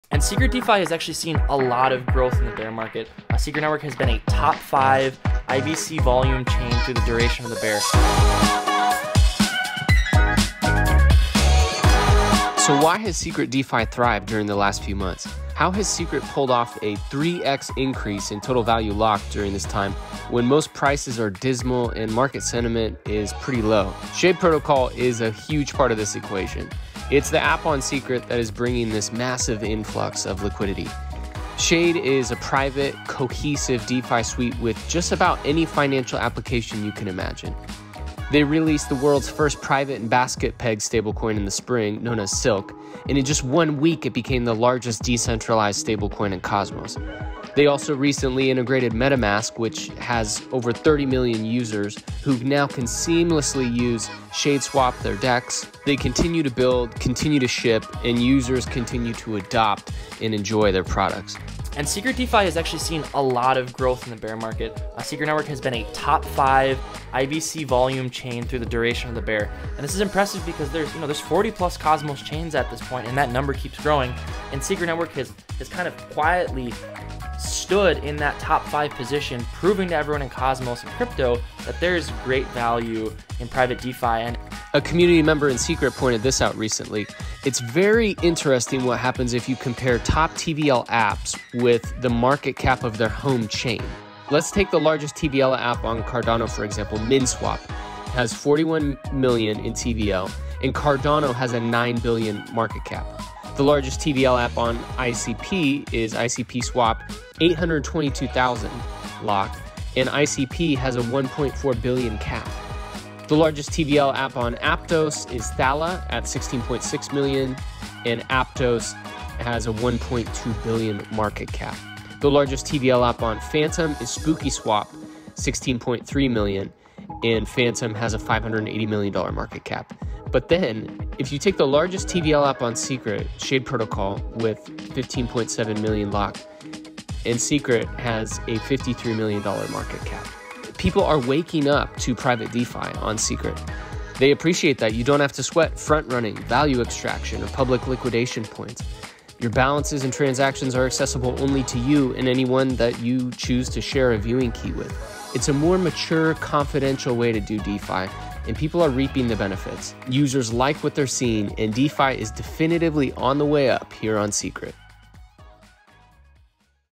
What is going on with Secret DeFi? (The REAL story)
The landscape of Secret DeFi has evolved rapidly during the last few months.

Here's a quick guided tour of what's going on and what could be next.